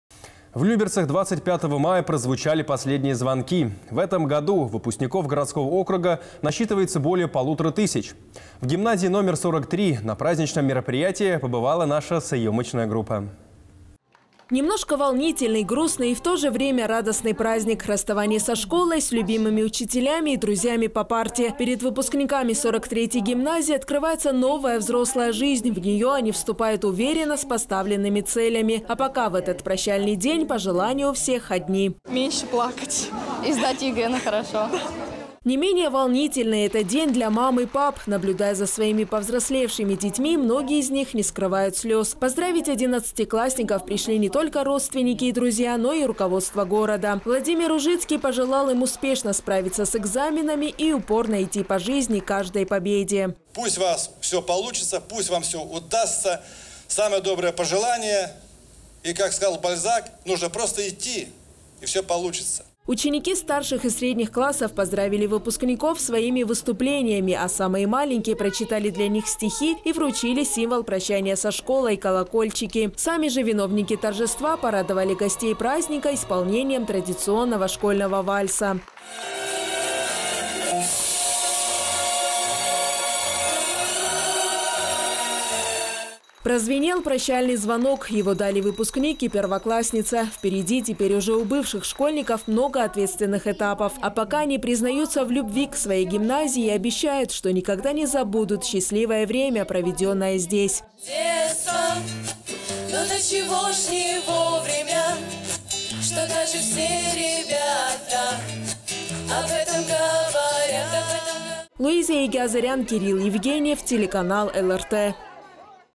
В Люберецких школах прошли последние звонки
«Прощай, школа». Эти слова произнесли более 1200 выпускников городского округа Люберцы. Для них 25 мая прозвенел последний звонок. С каким настроением одиннадцатиклассники покидают стены учебного заведения, узнавал наш корреспондент.

Немножко волнительный, грустный и в то же время радостный праздник. Расставание со школой, с любимыми учителями и друзьями по парте. Перед выпускниками 43 гимназии открывается новая, взрослая жизнь, в нее они вступают уверенно с поставленными целями. А пока в этот прощальный день – пожелания у всех одни:

"Меньше плакать и сдать ЕГЭ хорошо".

Не менее волнительный этот день для мам и пап, наблюдая за своими повзрослевшими детьми, многие из них не скрывают слез. Поздравить одиннадцатиклассников пришли не только родственники и друзья, но и руководство города. Владимир Ружицкий пожелал им успешно справиться с экзаменами и упорно идти по жизни к каждой победе.

Владимир Ружицкий, глава городского округа Люберцы:
"Пусть у вас все получится, пусть вам все удастся, самые добрые пожелания. И, как сказал Бальзак, нужно просто идти и все получится".

Ученики старших и средних классов поздравили выпускников своими выступлениями, а самые маленькие прочитали для них стихи и вручили символ прощания со школой – колокольчики. Сами же виновники торжества порадовали гостей праздника исполнением традиционного школьного вальса.

Прозвенел прощальный звонок, его дали выпускник и первоклассница. Впереди теперь уже у бывших школьников много ответственных этапов. А пока они признаются в любви к своей гимназии и обещают, что  никогда не забудут счастливое время, проведенное здесь.

Луиза Егиазарян. Кирилл Евгеньев